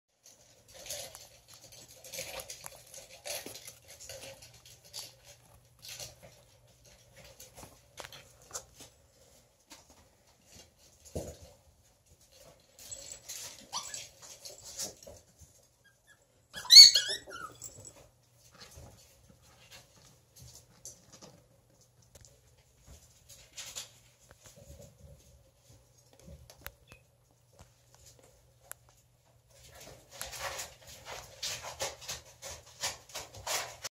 Germans in a Toy Box
König Von Übelriechend & Schneeflocke in the new toy box filled with their new toys.